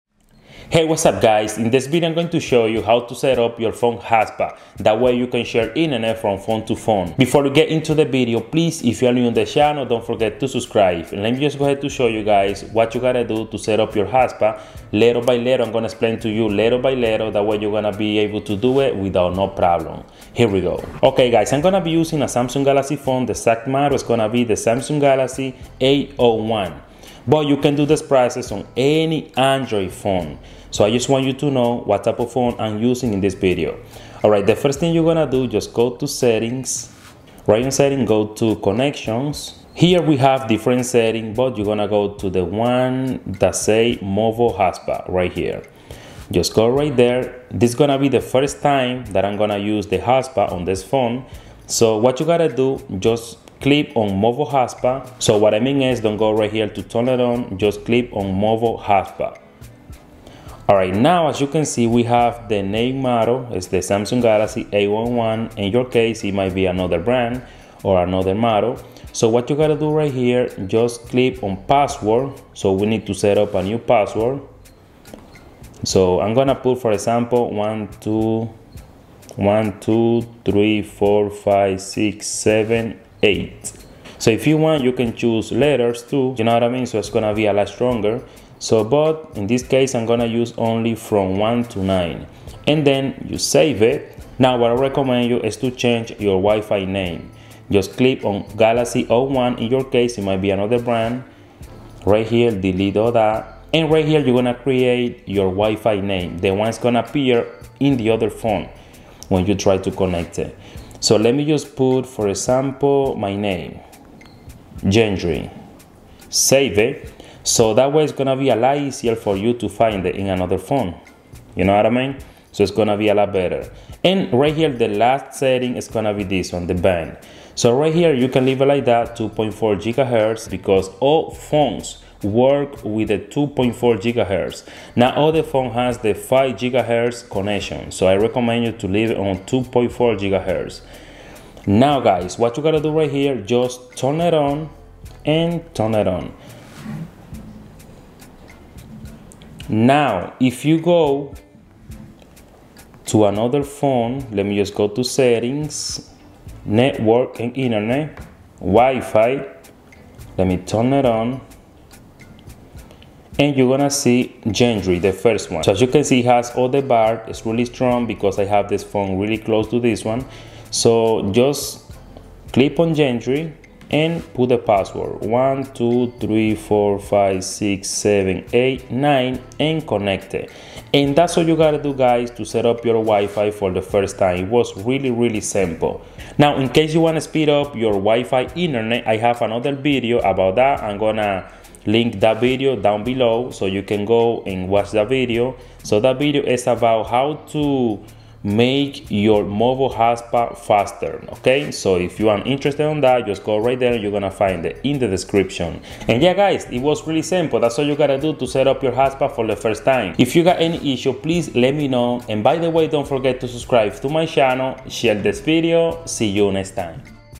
how to share wifi internet from phone to phone
How to use the hotspot on android/ in this video I show you how to set up the phone hotspot to share internet from phone to phone!!!